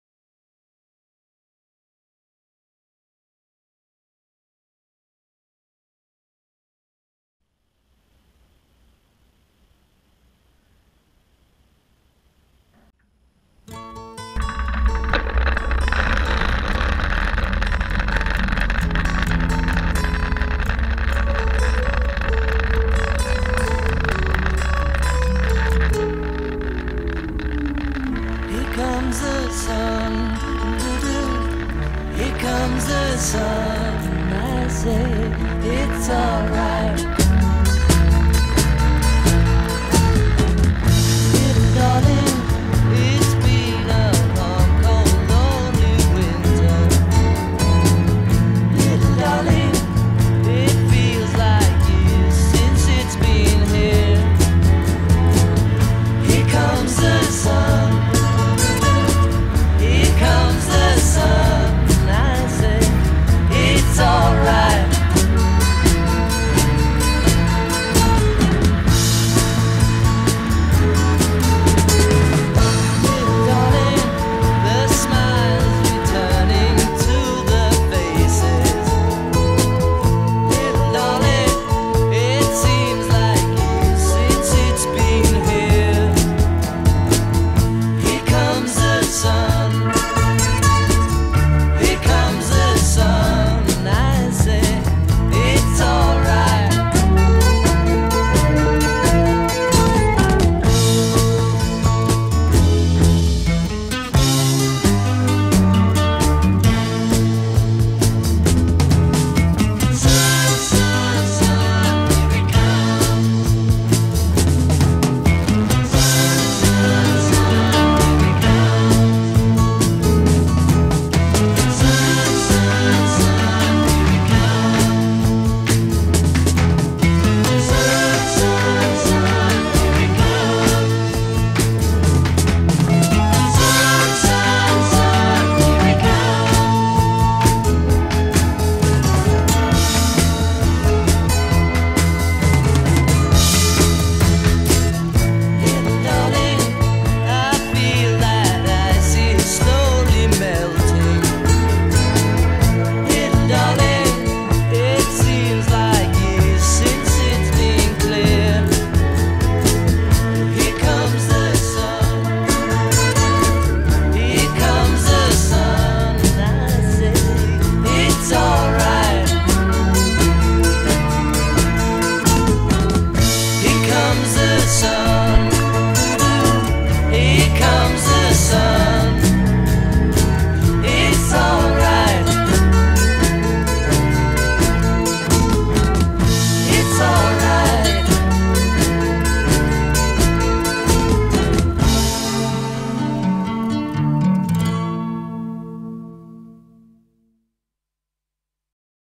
Father's Day - flying along the water 6-15-14
With Mackenzie at sunrise, New Smyrna Beach, Daytona Beach